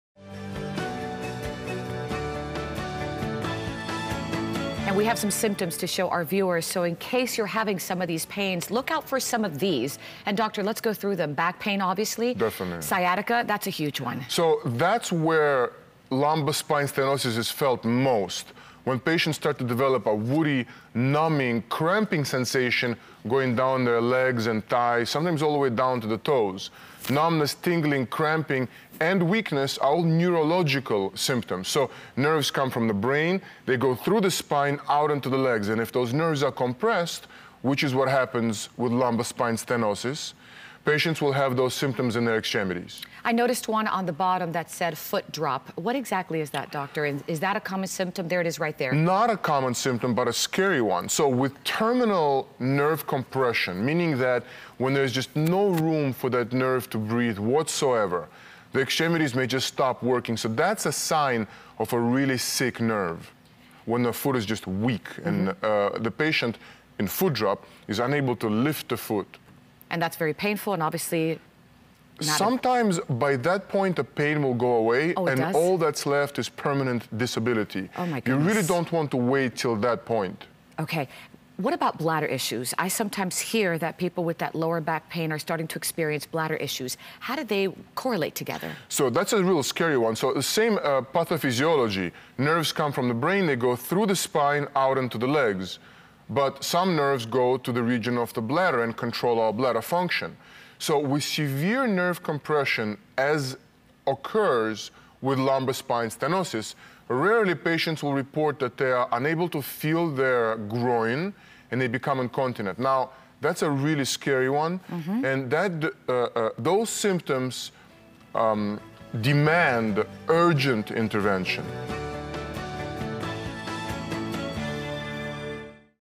Symptoms of Lumbar Stenosis
Back pain, sciatica (burning pain going into buttocks and down into legs), numbness, tingling, cramping, or weakness in legs, loss of sensation in feet, and foot drop are some symptoms of lumbar stenosis.

Dr. Georgiy Brusovanik, Spine Surgeon with Baptist Health South Florida, says numbness, tingling, cramping, and weakness are neurological symptoms. It means nerves come from the brain, they go through the spine out into the legs and if those nerves are compressed, the pain is going to happen.